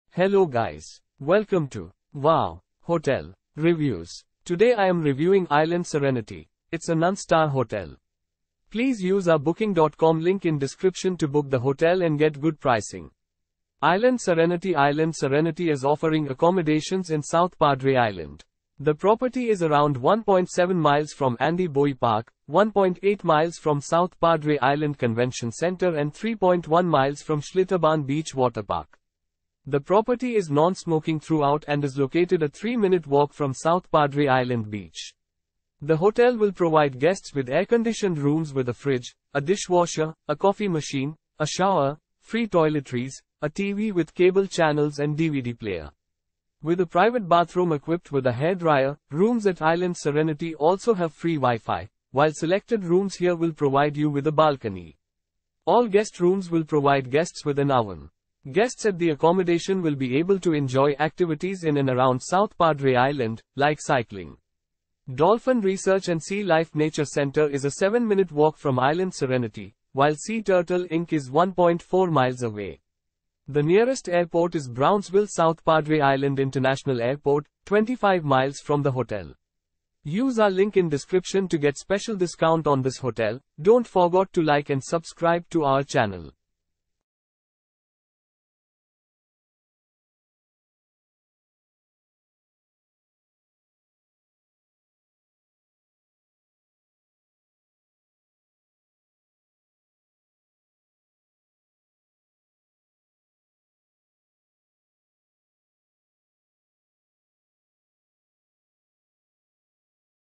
Island Serenity Review - South Padre Island , United States of America
Island Serenity

 Island Serenity
Island Serenity is offering accommodations in South Padre Island. The property is around 1.7 miles from Andy Bowie Park, 1.8 miles from South Padre Island Convention Center and 3.1 miles from Schlitterbahn Beach Waterpark. The property is non-smoking throughout and is located a 3-minute walk from South Padre Island Beach.

The hotel will provide guests with air-conditioned rooms with a fridge, a dishwasher, a coffee machine, a shower, free toiletries, a TV with cable channels and DVD player. With a private bathroom equipped with a hairdryer, rooms at Island Serenity also have free WiFi, while selected rooms here will provide you with a balcony. All guest rooms will provide guests with an oven.

Guests at the accommodation will be able to enjoy activities in and around South Padre Island, like cycling.

Dolphin Research and Sea Life Nature Center is a 7-minute walk from Island Serenity, while Sea Turtle Inc is 1.4 miles away. The nearest airport is Brownsville South Padre Island International Airport, 25 miles from the hotel.